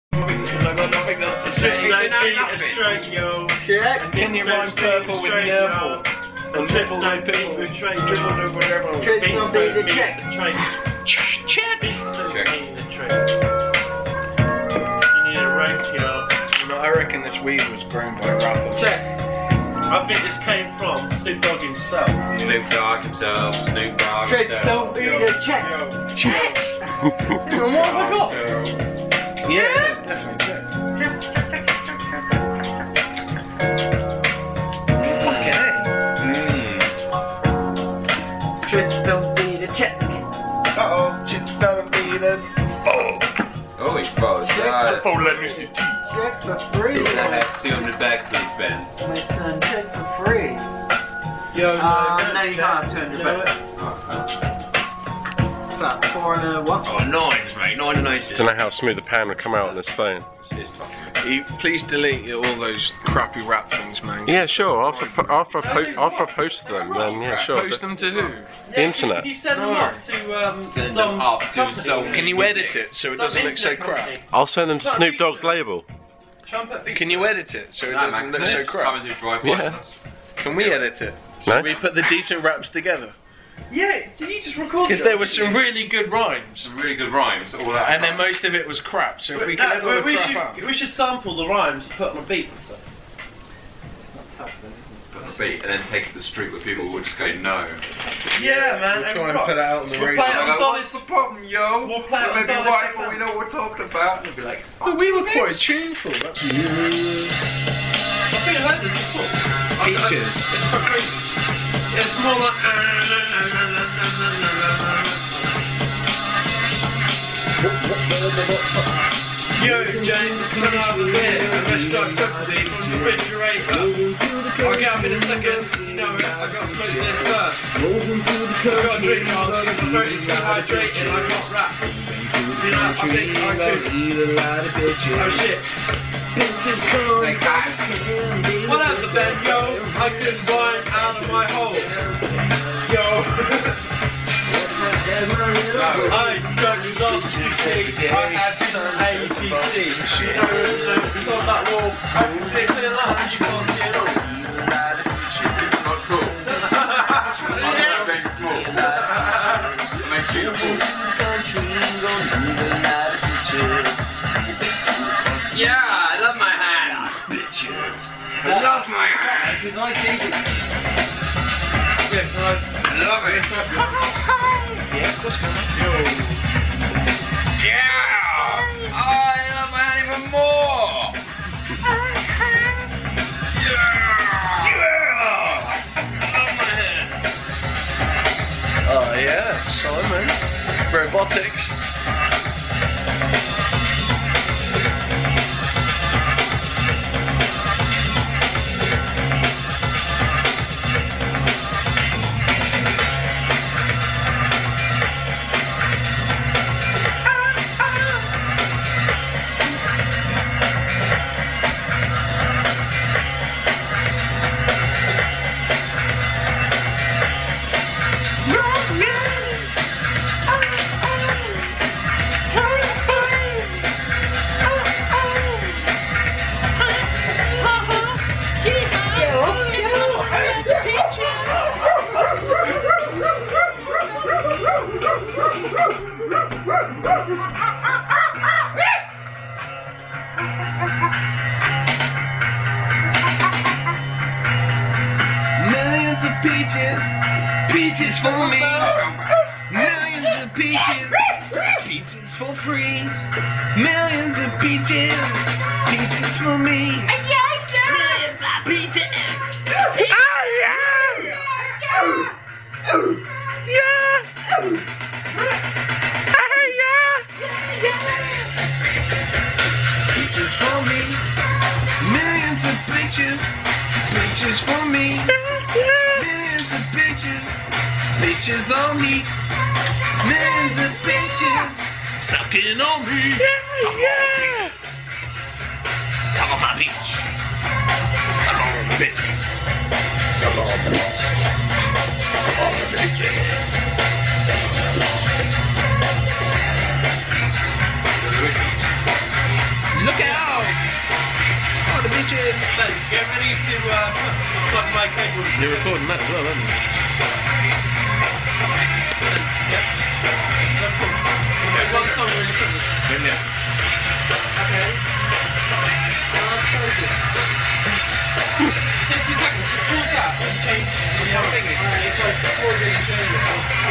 Trips Dont Beat A Check Yo
Five boys play poker and Rap.